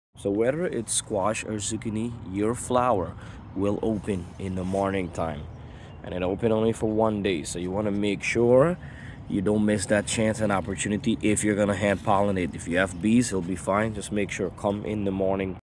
Time of day zucchini flowers open ! Same for squash plants courgette For Beginners
Zucchini flowers tend to open up wide in the morning and are often closed by the afternoon, so it is important to hand pollinate in the morning. Pluck a fully open male flower from the plant. Peel off the petals to expose the pollen-heavy anther.

growing zucchini plant
growing squash plant
growing Courgette Plant